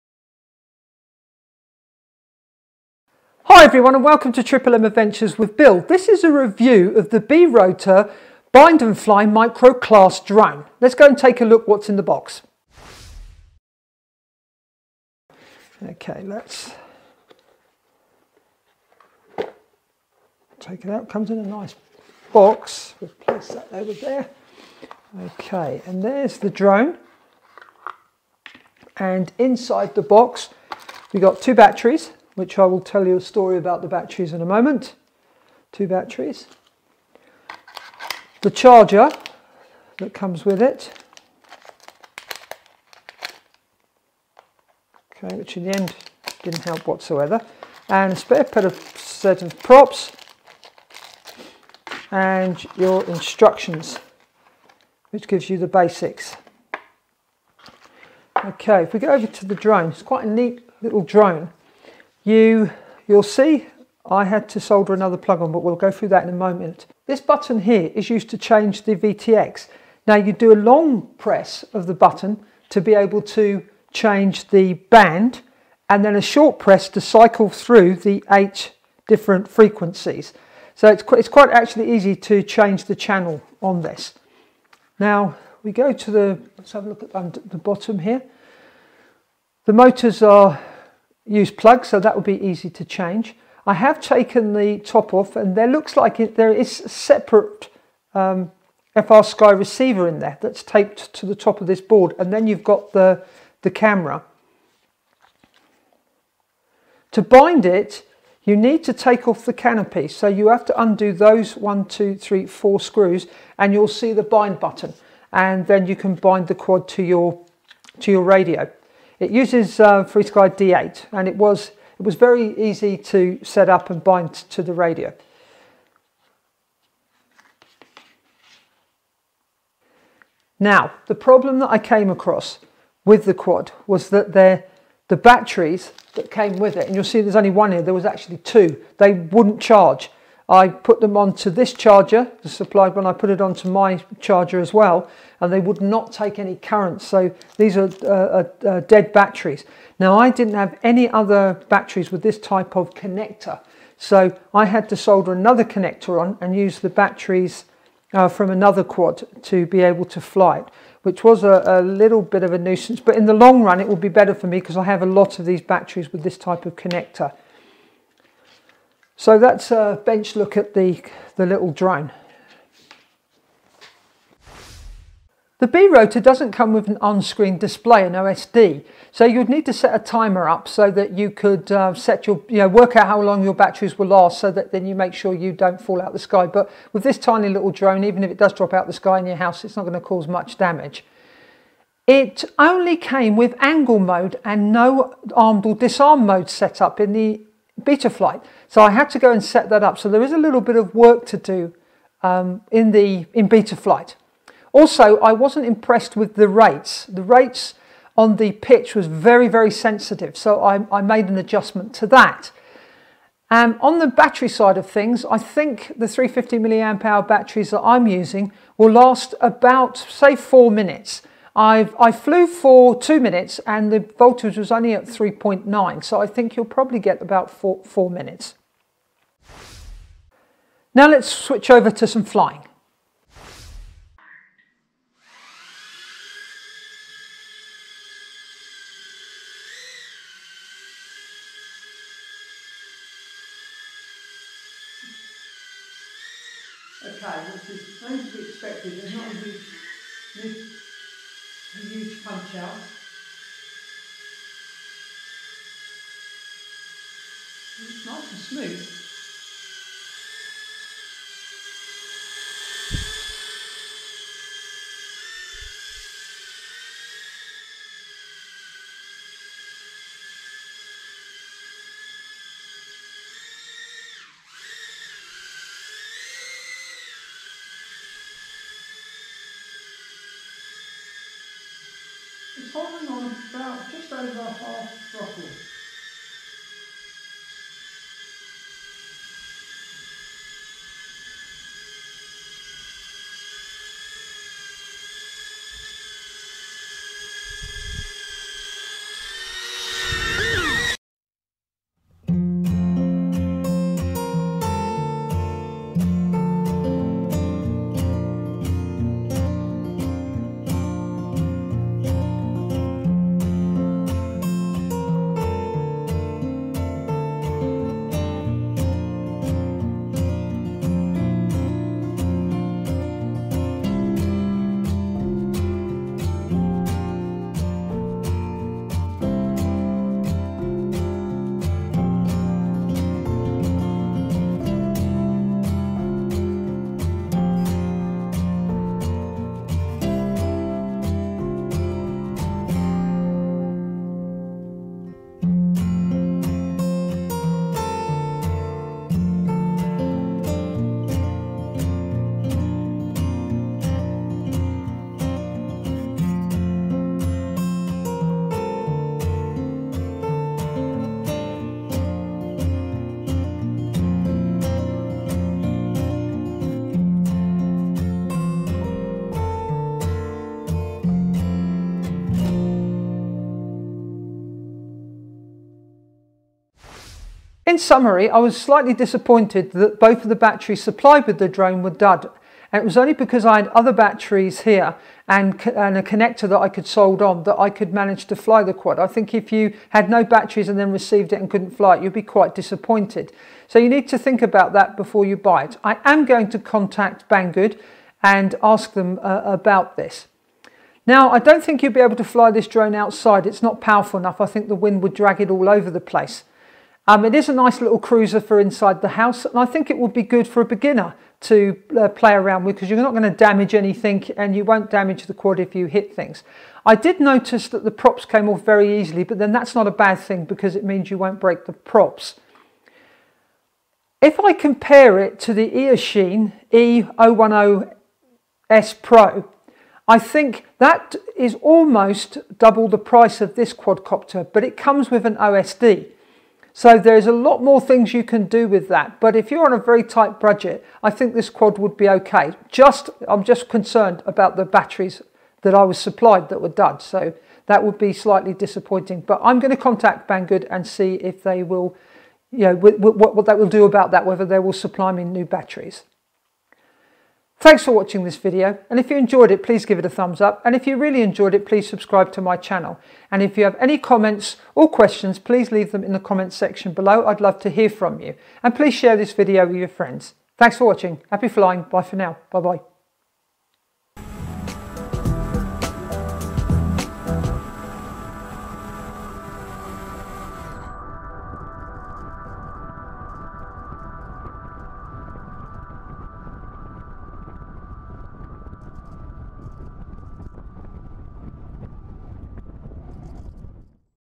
Review: BeeRotor BNF Micro Class Drone
This drone does not have an OSD (On Screen Display) It will struggle outside but will be a nice sedate quadcopter for flying indoors. It is not very powerful so will not do acro but great for days when you cannot fly outside with your bigger drones. The camera looks ok but you can make your own mind up from the flight footage in my video.

The downside with the one I purchased was both batteries where dead they would not charge pass 1.17 v! So I need to cut off the connector supplied and solder one that matched my batteries. I am sure this would be very disappointing if you received your BeeRotor and then could not fly it.

Another problem was that only Angle Mode was set up on the flight controller and no Arm / Disarmed mode so there is a little work to do when you get it. I also adjusted the rates as I thought the pitch was too sensitive for a small indoor drone.

 Would I recommend it, the batteries are a problem for me but that could have been my bad luck. For the price it’s ok and will allow you to practice FPV on rainy days when you cannot get outside. Is it for a beginner? Yes I think so but you will need to know how to set it up in Betaflight.

Please see link below if you would like to purchase the item. You will pay no more for the product, thank you.

I did purchase the BeeRotor BNF Micro Class Drone with my own money.

BeeRotor BNF Micro Class Drone

Happy flying.